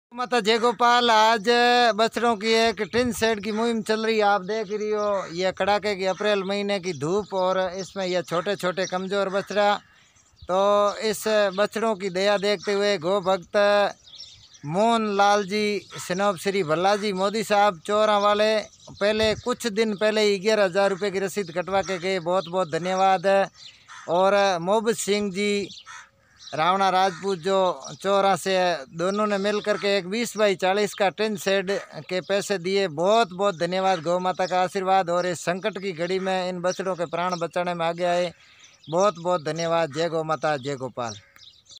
कोरोना जेसे संकट में भी गो माता के लिए आगे आये गो भग्त धन्य हैं ऐसे गो भग्तो के माता पिता को
गो धाम चौरा राजस्थान का ऐक मात्र छोटे बछड़ो का गो शाला केवल सोसल मिडिया के माध्यम से आ रहा है सयोग प्रति दिन लाईव सेवा गो भग्तो ईस सकंट मे सयोग जरूर करें जयादा जानकारी के लिए फोन करें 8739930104